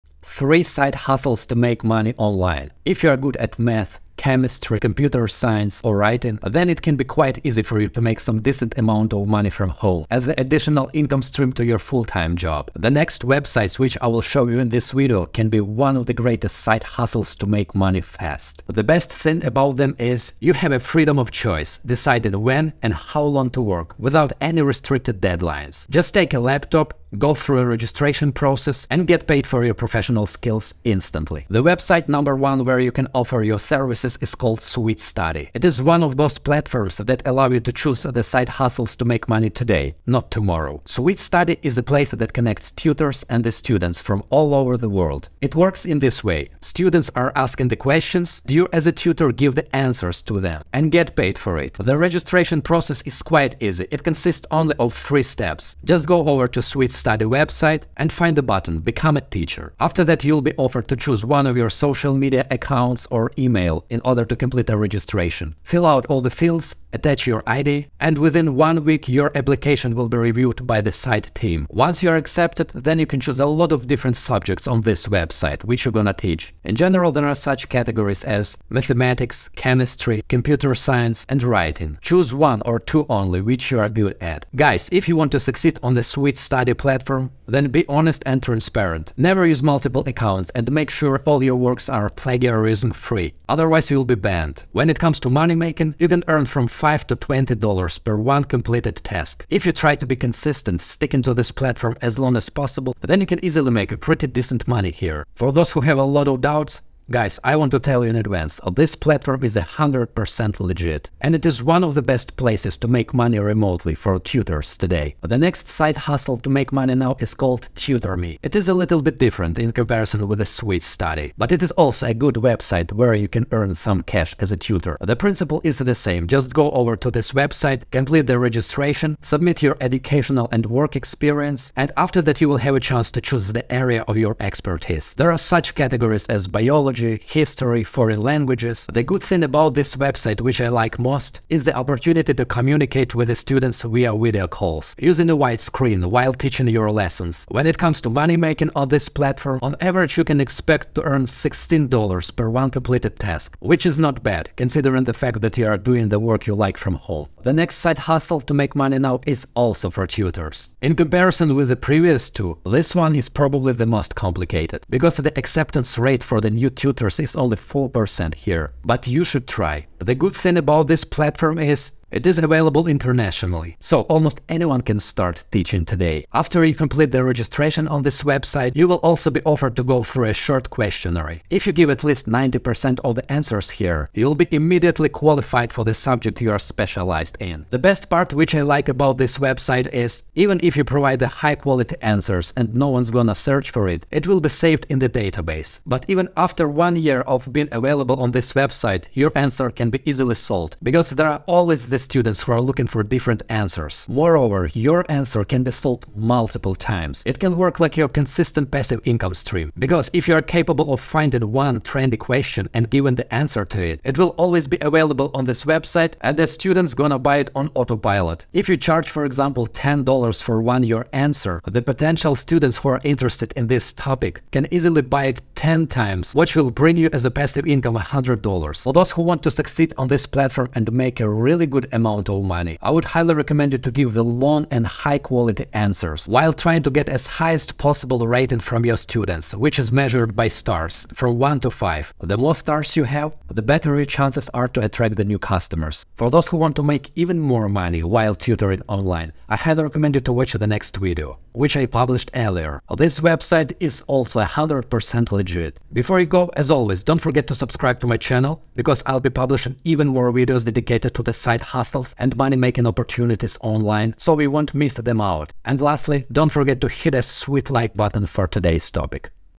Side Hustles to Make Money Online
3 Side Hustles to Make Money Online. In this video you will know three good
side hustles to make money fast. They can be ideal for those who are looking for opportunities of earning some extra income from home.

The first web-site  in our list is called SweetStudy. It is definitely one of the best side hustles to make money today. SweetStudy is a tutoring platform that connects many students and teachers online. Your main goal is to provide the proficient answers to the specific questions on different subjects such as: mathematics, chemistry, business and finance etc.

One can easily make from 5-20 dollars per one completed task here.

The next site that gives you an opportunity to earn cash remotely is called Tutorme. It is also considered as one of the best side hustles to make money from home. After having completed the registration, choose the area of your expertise. Then you'll be allowed to tutor the students via video calls.

Regarding a salary one can earn on this platform, expect to make around $16/hour.

The last good side hustle which you can do from home is a site called School Solver. This one is a little bit different in comparison with the previous two. Cause it requires you to give more profound answers providing a full explanation to the questions. But no one will see them completely (only a preview) until they pay you for them.

Eventually, your success will be measured by the stars (from 1-5) on School Solver. The more students give you the scores for one assignment, the more site trust and the new clients you will be able to attract.

** All video and audio content created by myself